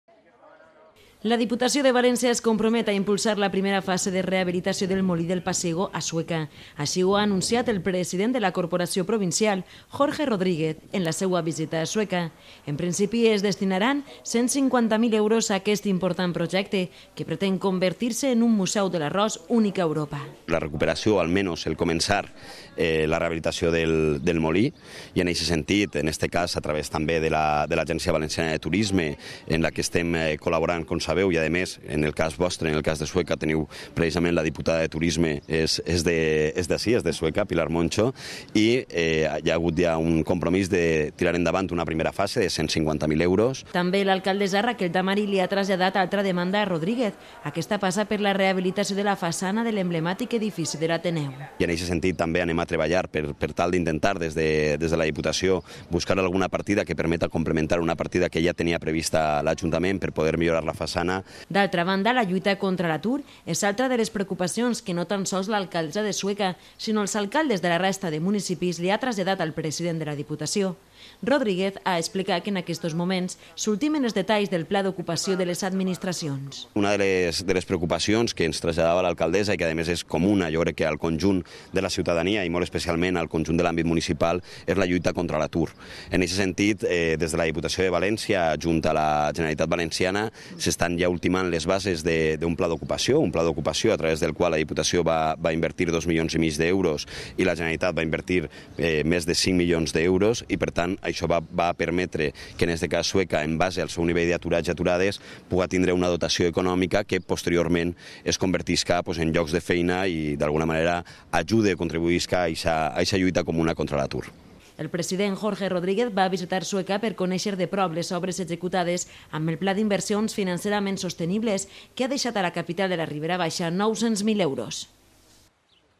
Diputació es compromet a impulsar la primesa fase de rehabilitació del Molí del Pasiego
L’Alcladessa de Sueca, Raquel Tamarit, li trasllada distintes demandes  al president de la Diputació, Jorge Rodríguez.